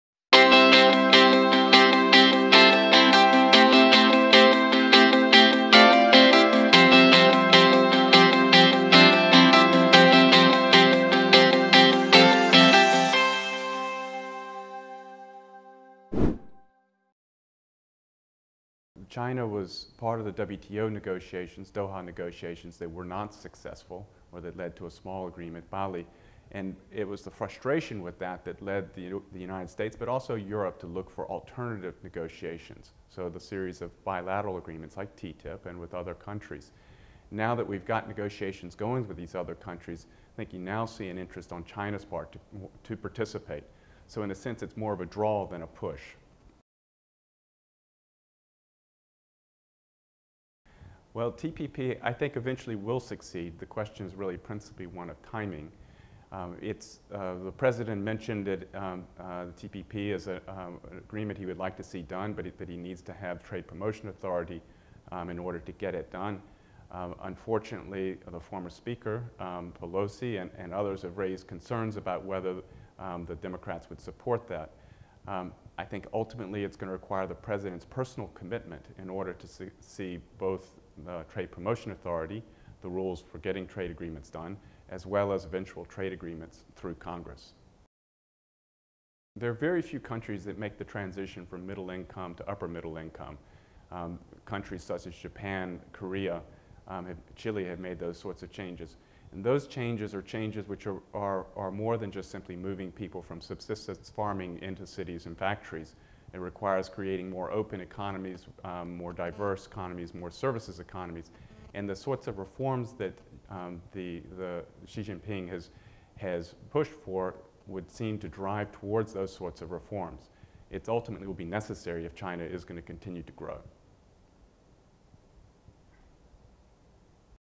Stockholm China Forum 13: Rod Hunter Discusses Economic Relations between U.S. and China
Rod Hunter discusses the U.S. administration's major trade initiatives such as TTIP and TPP and their effect on economic relations with China. He also discusses the effect Xi Jinping's new economic reforms might have on China.

Rod Hunter, Senior Vice President, PhRMA